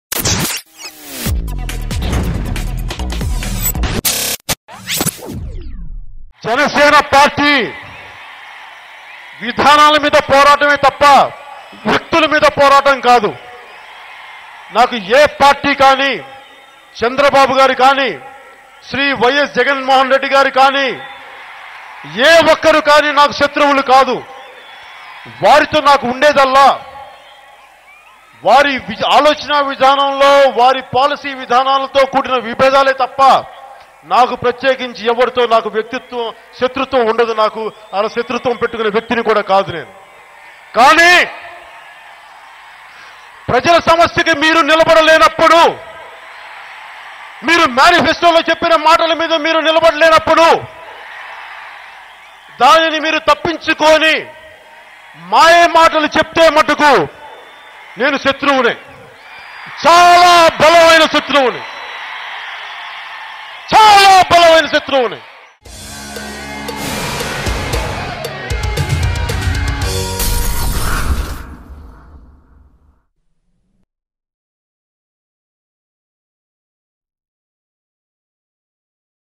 Pawan Kalyan Calls Himself a Powerful Enemy | Jana Sena Party Public Meeting at Anantapur | Janasena
Pawan Kalyan Calls Himself a Powerful Enemy at Jana Sena Party public meeting at Anantapur, on Telugu Filmnagar. Janasenaparty president PawanKalyan fires on the AP state government.